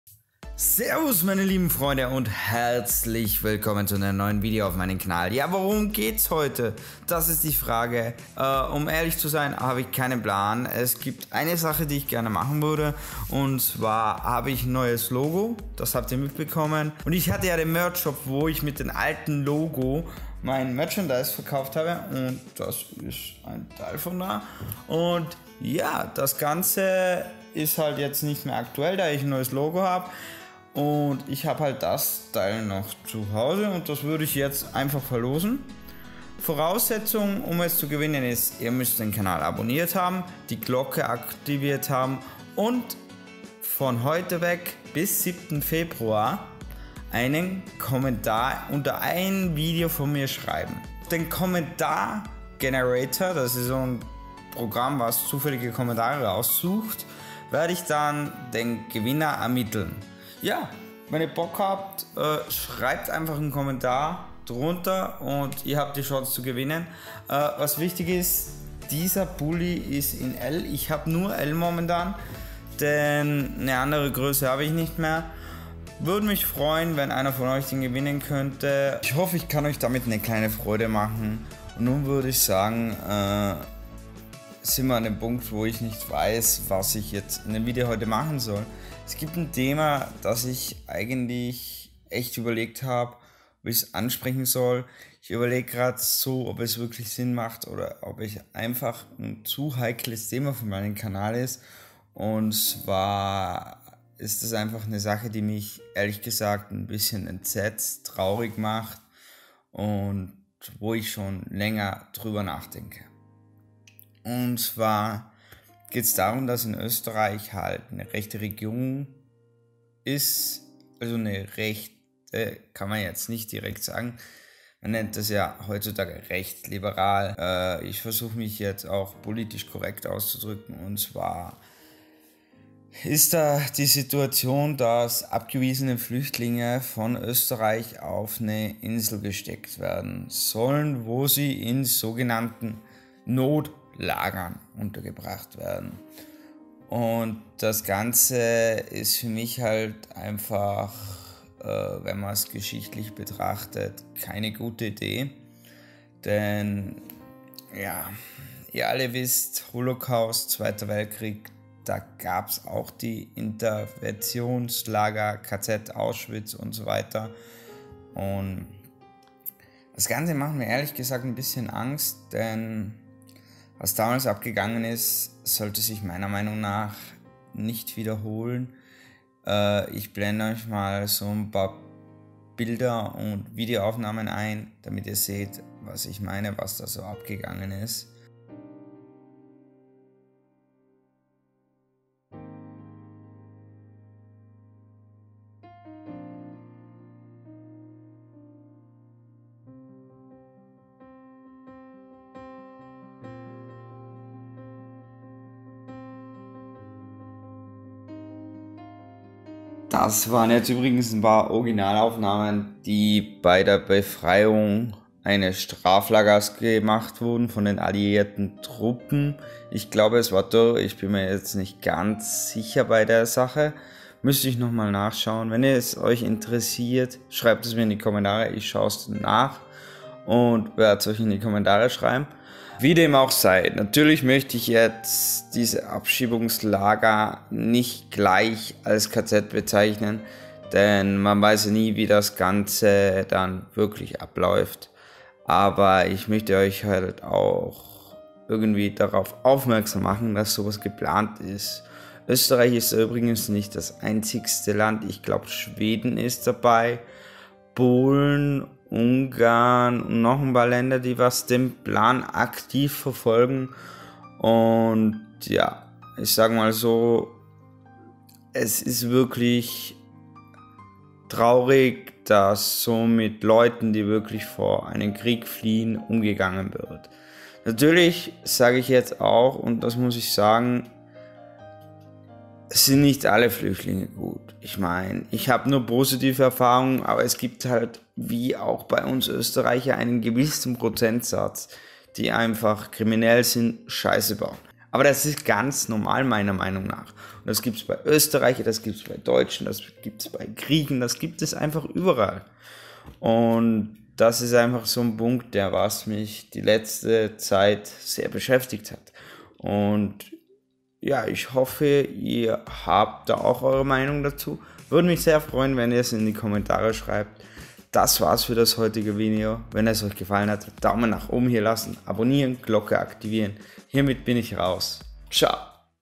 Österreich plant Insel Lager für Flüchtlinge / Verlosung
Das Flüchtlings Thema beginnt ab 01:30min wenn die Verlosung nicht interessiert bitte skipen.

► MEIN EQUIPMENT ◄
* Kamera: Canon 7D
* Stativ: Hähnnel
* Objektiv: Sigma 10-20 mm Weitwinkel
* Beleuchtung:Walimex Softbox